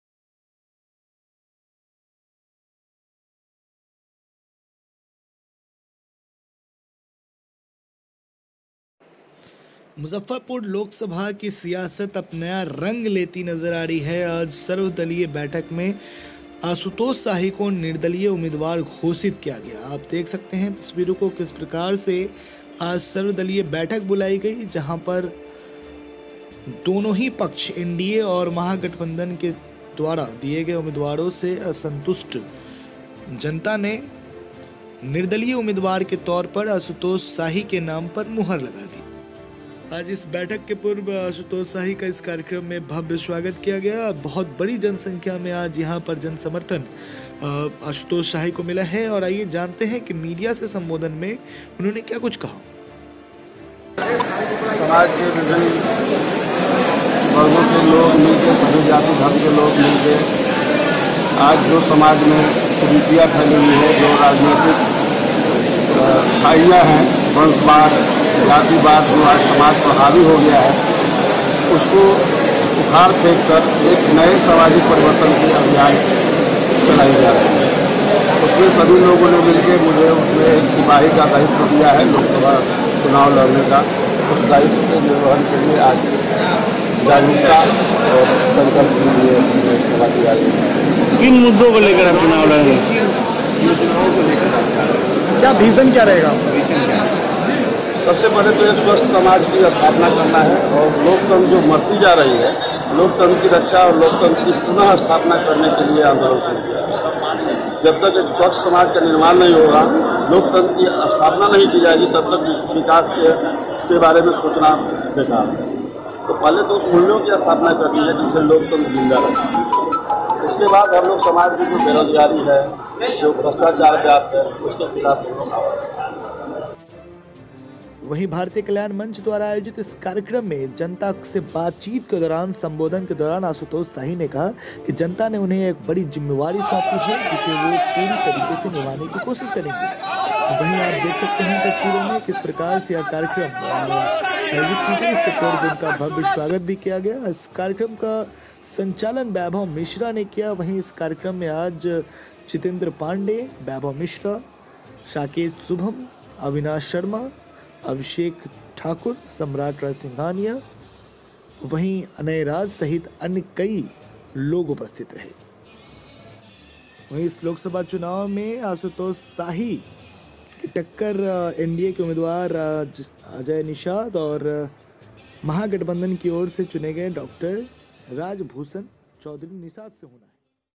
Ashutosh Shahi | Independent Candidate | Muzaffarpur Loksabha 2019
मुज़फ़्फ़रपुर लोकसभा चुनावी जंग में अशुतोष शाही की हुई इंट्री-
- नगर के सियासत के बदलते तेवर, स्वर्ण समाज हुए एकजुट/अशुतोष शाही को चुना स्वर्ण समाज का नेता।

पिछले 05 साल से नगर के बदहाल स्तिथि को देखते हुए भारतीय कल्याण मंच के बैनर तेल आज स्वर्ण समाज की बैठक में अशुतोष शाही को समाज का नेता सुनिश्चित किया गया वही उन्हें लोकसभा चुनाव का उम्मीदवार भी घोषित किया गया।

शाही ने मंच से सभा को संबोधित करते हुए कहा कि मुझे जनता ने चुना हैं यह लड़ाई मेरी नही जानता कि हैं, मैं तो बस एक सच्चे सेवक की तरह समाज के सेवा को तत्पर हूँ।
मुज़फ़्फ़रपुर की विकास की गति जिस तरीके से एक षड्यंत्र के तहत धीमी कर दी गयी हैं अगर जनता ने विश्वास बनाये रखा और मझे मौका मिला तो निश्चित तौर पर इस लड़ाई को सफल बनाऊंगा।
इस दौरान नगर के प्रबुद्ध वर्ग के काफी लोग उपस्थित रहें- जिसमें मुख्य रूप से जितेंद्र पांडेय, वैभव मिश्रा, संजय चौधरी, साकेत शुभम, अविनाश आदि की भूमिका रहीं।

अशुतोष शाही | निर्दलीय उम्मीदवार | मुज़फ़्फ़रपुर लोकसभा 2019